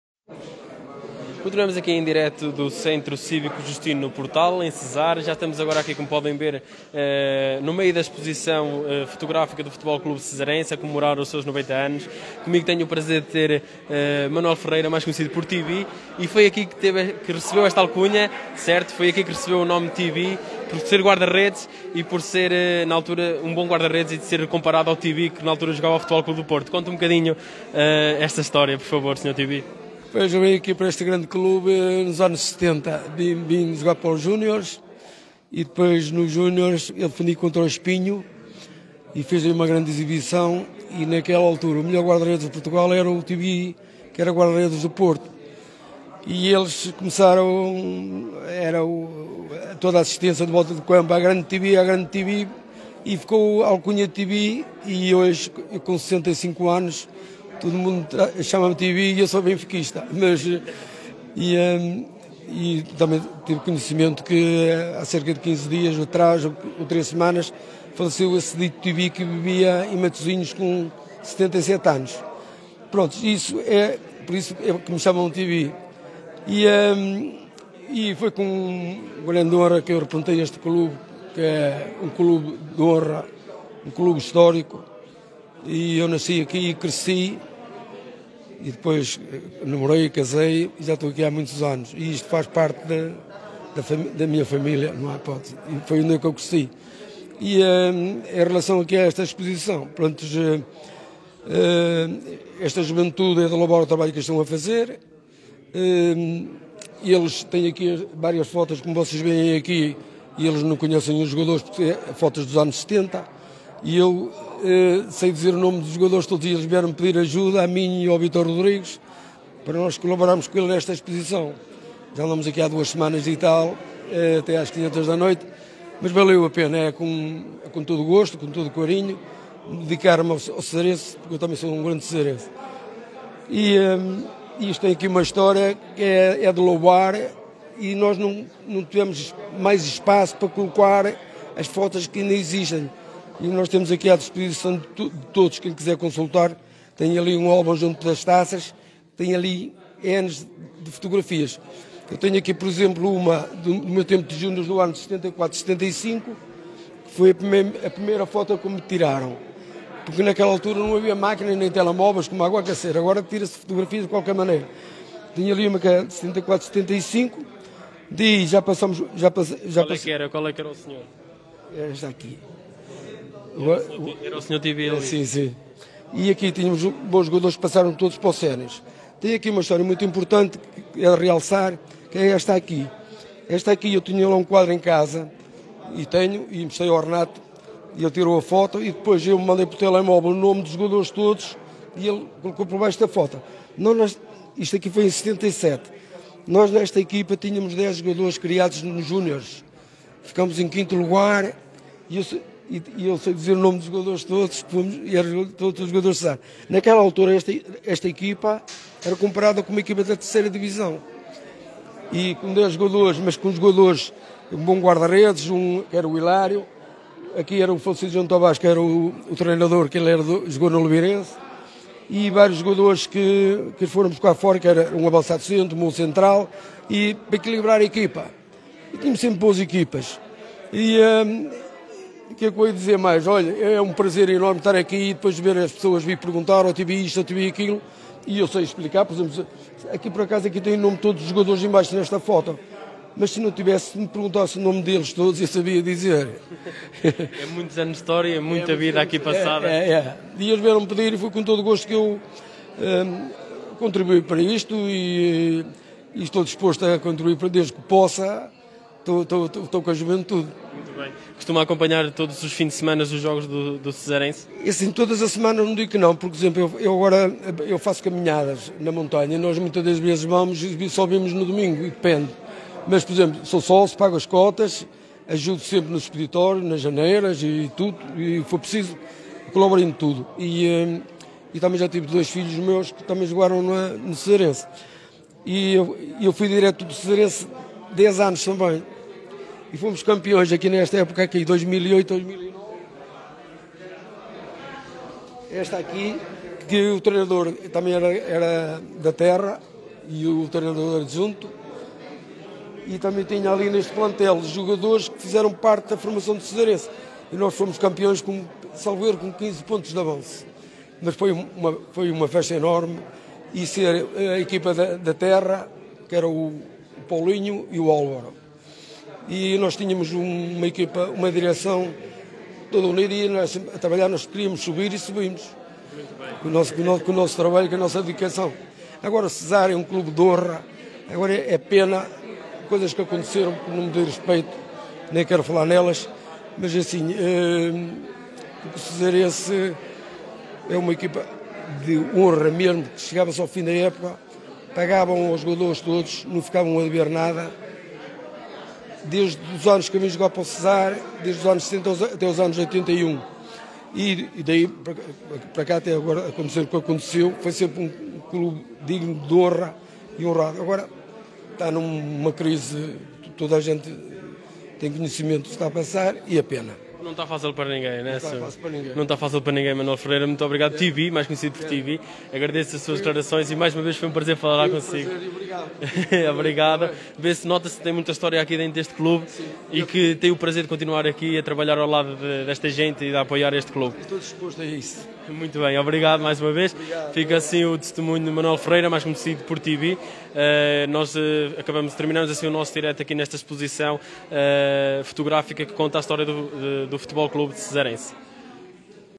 EXPOSIÇÃO FOTOGRÁFICA REPRESENTATIVA DA HISTÓRIA DO FUTEBOL CLUBE CESARENSE
No âmbito da comemoração dos 90 anos do FC Cesrense, a Comissão Organizadora e os Órgãos Sociais do clube inauguram uma exposição fotográfica representativa da sua história.A Azeméis TV acompanhou essa inauguração e recolheu as reações mais importantes.
Opinião de Manuel Ferreira 'Tibi'.